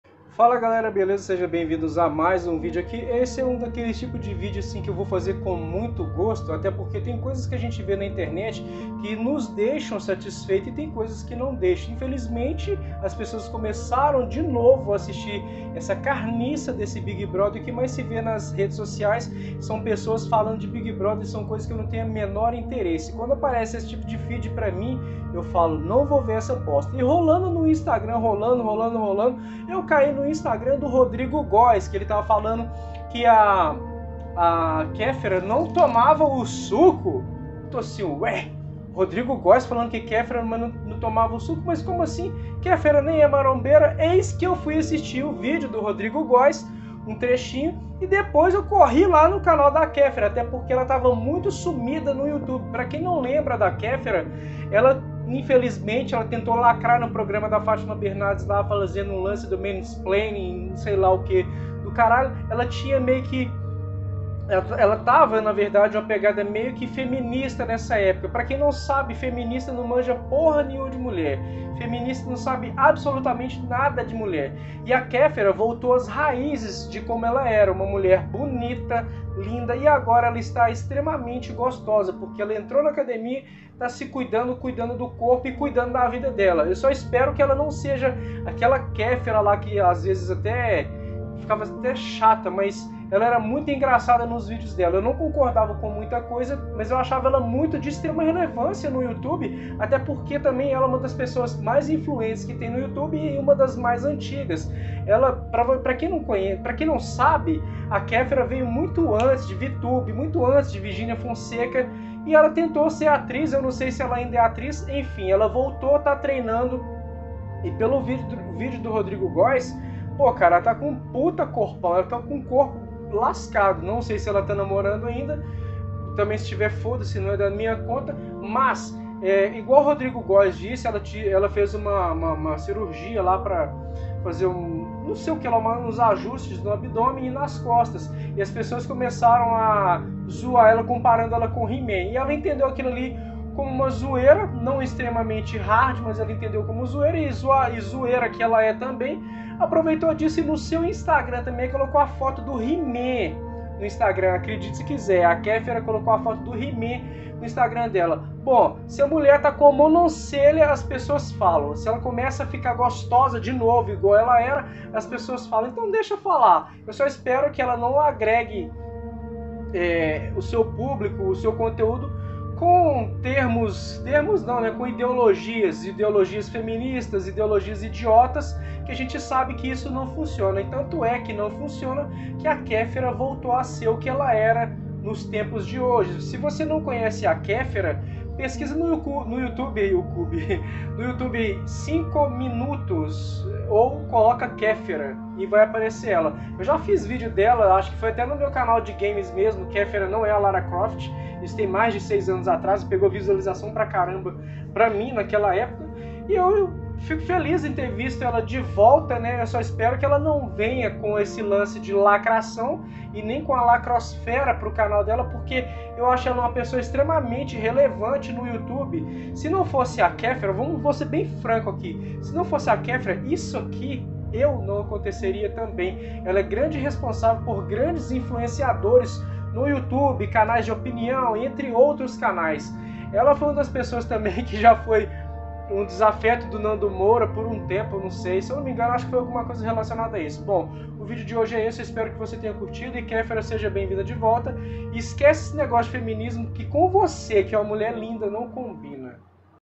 kEFERA DEIXA DE SER FEMINiSTA E É COMPARADA AO HE-MAN
Kefera é uma YouTuber das antigas que virou atriz , no entanto ela voltou ao YouTube para gravar vídeos .

Kefera era a Rainha dos YouTubers teen da época .